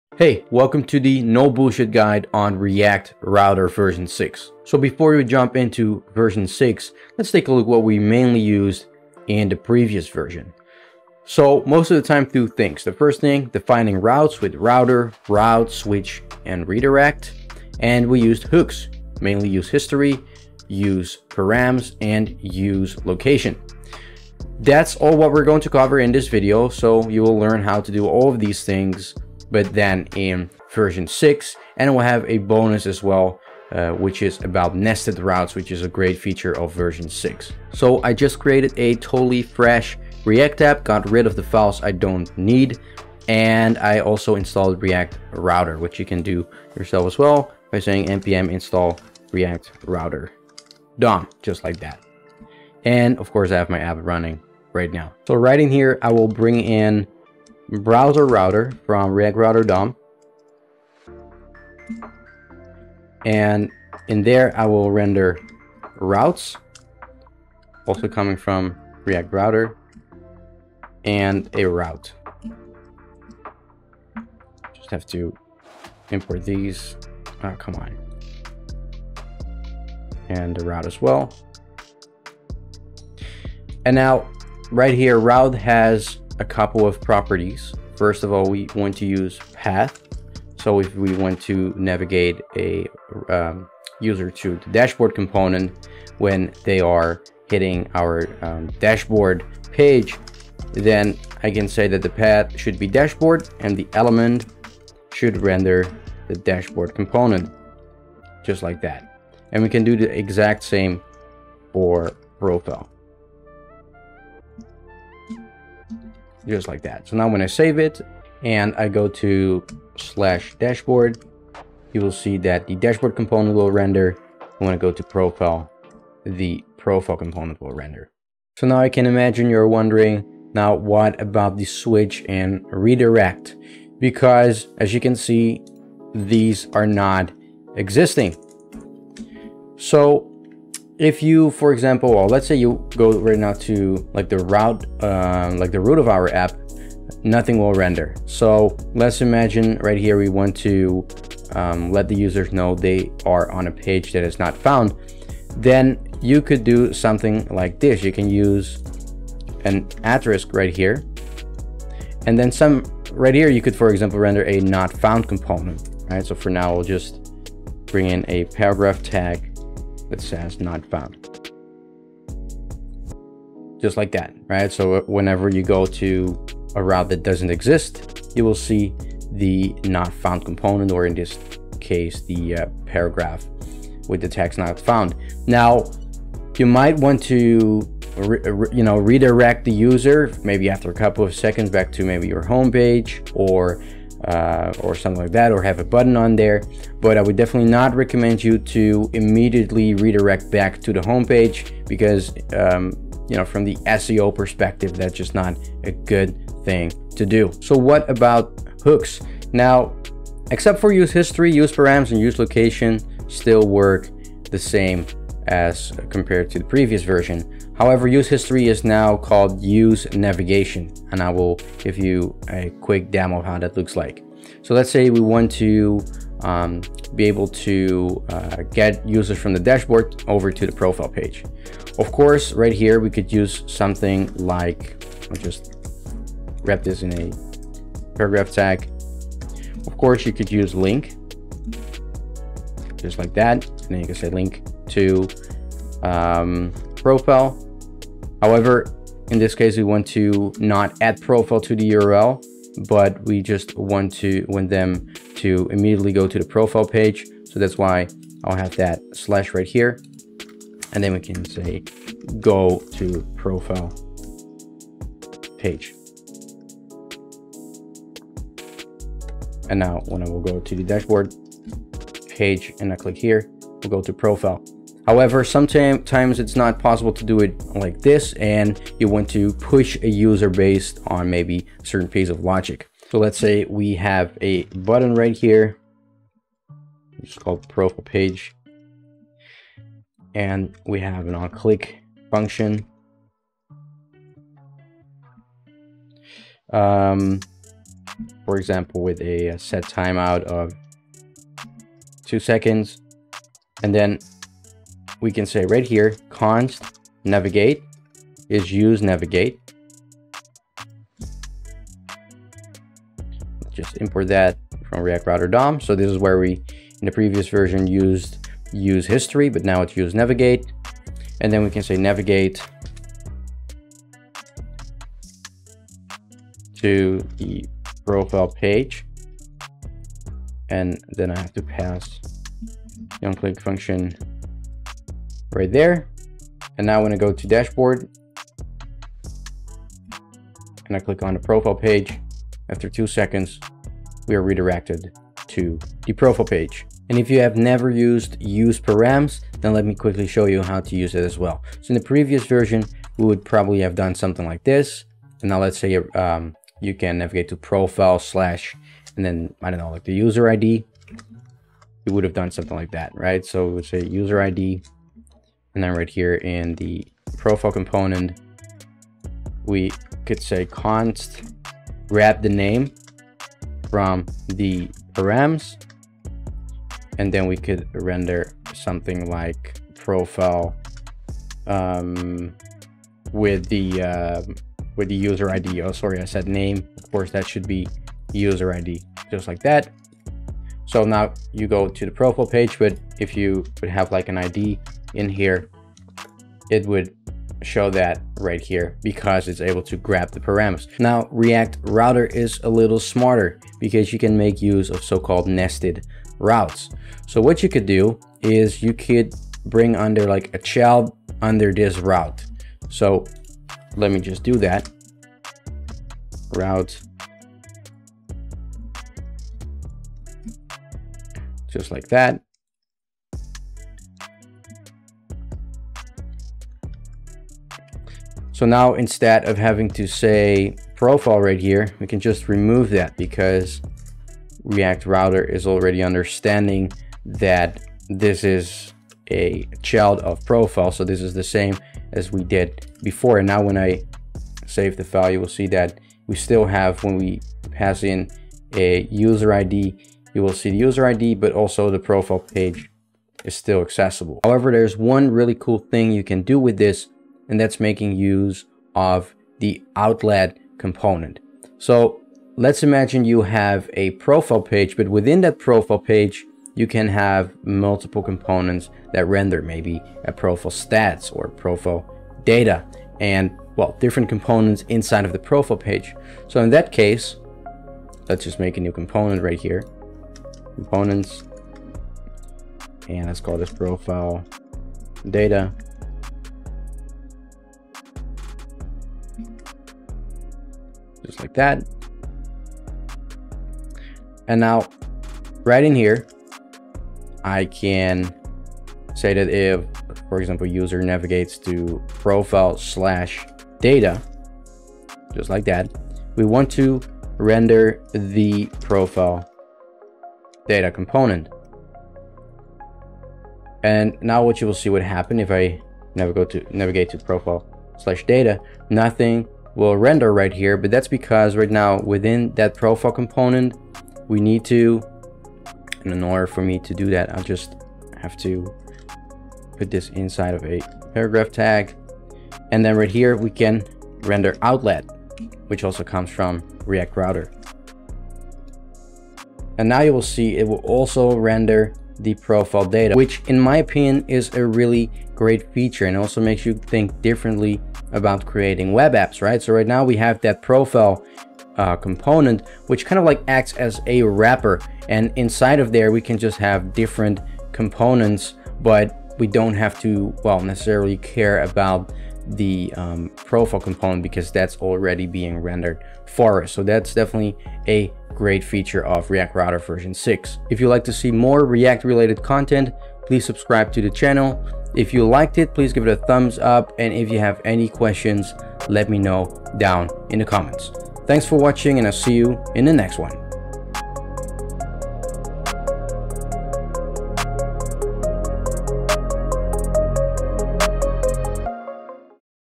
React Router V6 - the no BS Guide
Take your apps to the next level with these great tools for React Developers!

0:00 Intro
0:04 What did we use in V5
0:37 Defining routes
3:27 React Router hooks
6:40 Bonus: nested routes
9:20 Outro